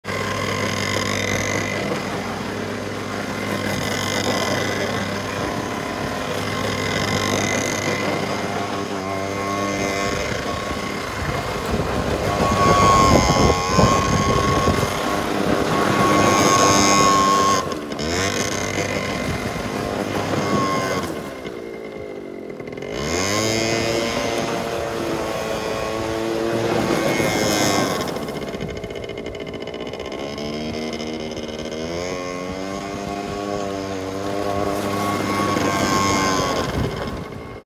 kids atv bombardier ds 50 can am
Ds50 drifting 6 year old